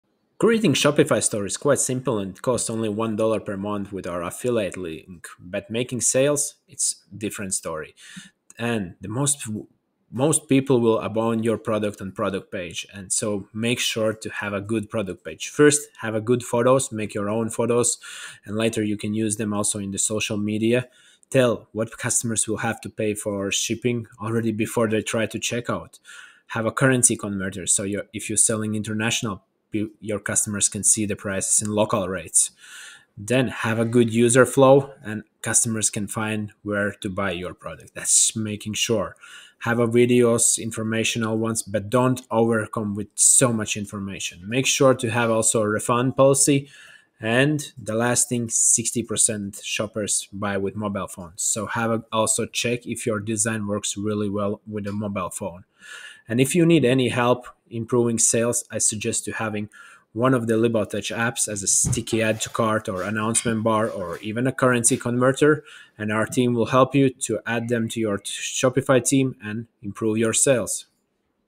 The Surprising Reason Customers Abandon Your Shopify Store
Learn valuable tips and strategies for creating a successful Shopify store in this viral video.  Find out how to improve user flow and make it easy for customers to purchase your products.